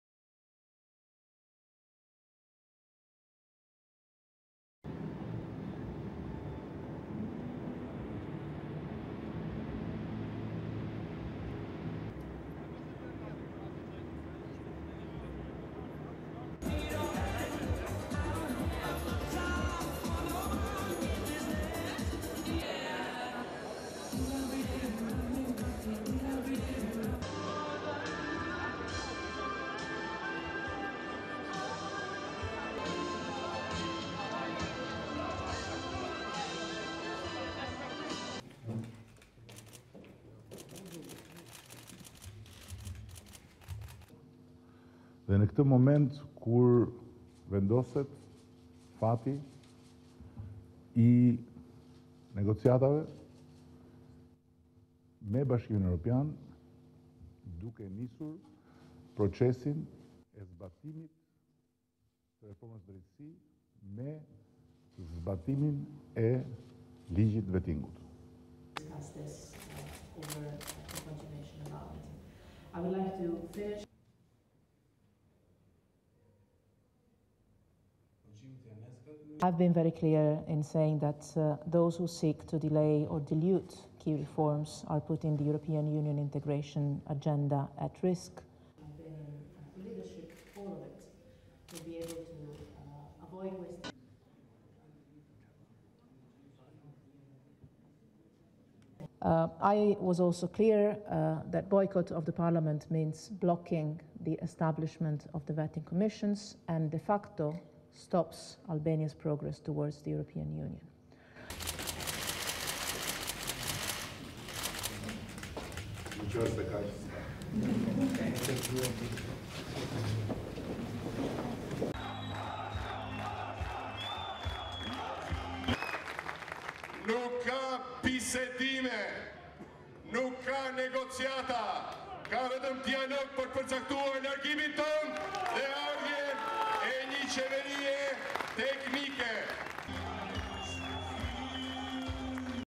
Albania's EU talks depends on Judiciary Reform and Vetting Implementation
- Rows over reforms and election bog down Albania's EU talks. Albania's opposition party say no to negotiations in a row over the running of the upcoming elections, stalling justice reform that is a key condition set by the EU for starting accession talks.

SHOWS: TIRANA, ALBANIA (MARCH 2, 2017) (REUTERS - ACCESS ALL)

1. EXTERIOR OF PRIME MINISTER'S OFFICE / BIG TENT OUTSIDE PM'S OFFICE WHERE OPPOSITION PROTESTERS GATHERED

2. POLICE STANDING, PEOPLE WALKING BY

3. VARIOUS OF OPPOSITION PROTESTERS GATHERED INSIDE TENT

4. PROTESTERS HOLDING DEMOCRATIC PARTY FLAG

TIRANA, ALBANIA (MARCH 3, 2017) (REUTERS - ACCESS ALL)

5. ALBANIAN PRIME MINISTER, EDI RAMA, AND EUROPEAN UNION FOREIGN POLICY CHIEF, FREDERICA MOGHERINI, ARRIVING AT NEWS CONFERENCE

6. (SOUNDBITE) (Albanian) ALBANIAN PRIME MINISTER, EDI RAMA, SAYING:

"At this moment when the fate of negotiations with the European Union is being decided, we are starting to implement the reforms in justice system and the implementation of the "vetting" law."

7. NEWS CONFERENCE IN PROGRESS

8. GUARDS OF HONOUR STANDING NEXT TO ALBANIAN FLAG

9. (SOUNDBITE) (English) EUROPEAN UNION FOREIGN POLICY CHIEF, FREDERICA MOGHERINI, SAYING:

"I have been very clear in saying that those who seek to delay or dilute key reforms are putting the European Union integration agenda at risk."

10. MOGHERINI SPEAKING

11. PHOTOGRAPHERS TAKING PICTURES

12. (SOUNDBITE) (English) EUROPEAN UNION FOREIGN POLICY CHIEF, FREDERICA MOGHERINI, SAYING:

"I was also clear that boycott of the parliament means blocking the establishment of the (judicial) vetting commissions and de facto stops Albania's progress towards the European Union."

13. MOGHERINI AND RAMA SHAKING HANDS AND LEAVING

TIRANA, ALBANIA (MARCH 4, 2017) (REUTERS - ACCESS ALL)

14. HEAD OF DEMOCRATIC PARTY, LULZIM BASHA, ENTERING THE OPPOSITION TENT, PEOPLE CHANTING

15. (SOUNDBITE) (Albanian) HEAD OF DEMOCRATIC PARTY, LULZIM BASHA, SAYING:

"There will be no talks, no negotiations, there will be just a dialogue to determine your (Edi Rama) departure and the arrival of a technocrat government, (until new elections)."

16. BASHA SHAKING HANDS WITH PEOPLE

STORY: The leader of Albania's main opposition Democratic Party, Lulzim Basha said on Saturday (March 4) there would be no talks and no negotiations in a row over the running of the upcoming elections, stalling the justice reform and holding back European Union membership talks.

Several thousand members of the Democratic Party (DP) and its lawmakers have camped out in an 800-square-metre (955 square yards) tent in front of Albanian Prime Minister Edi Rama's office. They are demanding a technocratic government to take over before free parliamentary elections in June.

The two-week-long protest and its boycott of parliament have stalled the creation of bodies that would vet 750 judges and prosecutors. Reform of the judiciary is a key condition set by the European Union for starting accession talks.

The ruling coalition had offered to sit down for talks with the Democrats ahead of a visit of EU's foreign policy chief Federica Mogherini on Friday (March 3).

At the news conference in Tirana, Mogherini told NATO member Albania it had a real chance of clinching accession talks once the vetting process started.

"Boycott of the parliament means blocking the establishment of the (judicial) vetting commissions and de facto stops Albania's progress towards the European Union," she said.

Her comments were rejected by Democratic Party head Lulzim Basha, who joined the protesters in the tent.

The opposition blames the socialist government of failing to deliver 300,000 promised jobs and free health care, also accusing it of corruption and links to crime and drug traffic.

Albania is expected to grow a respectable 3.8 percent this year according to forecasts from the International Monetary Fund. The country is facing a political deadlock over justice system reforms ahead of parliamentary elections in June.